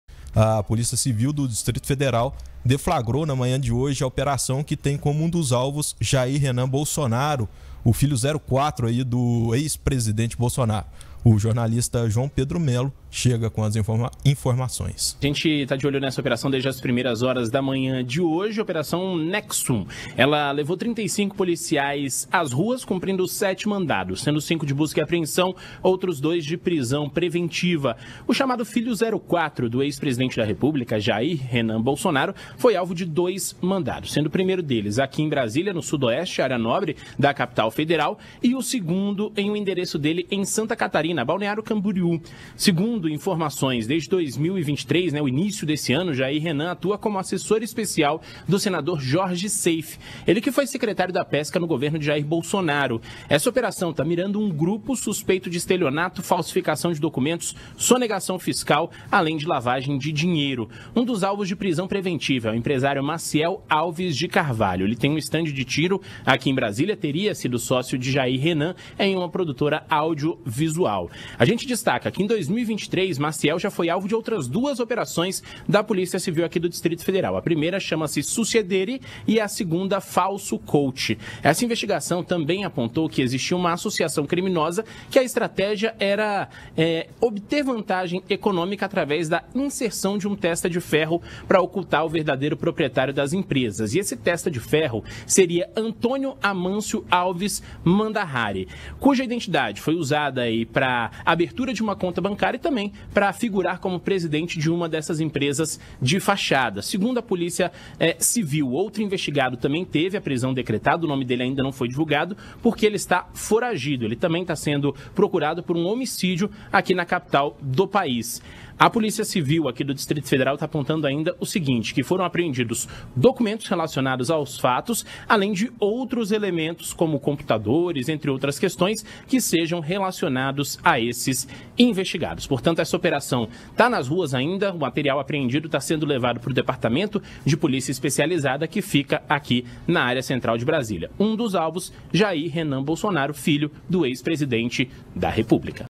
JAIR RENAN BOLSONARO É ALVO DE MANDADOS DE BUSCA E APREENSÃO
| BRASIL |

A Polícia Civil do Distrito Federal deflagrou na manhã de hoje, a operação Nexum que tem como um dos alvos Jair Renan Bolsonaro, um dos filhos do ex-presidente. O jornalista João Pedro Melo traz mais informações.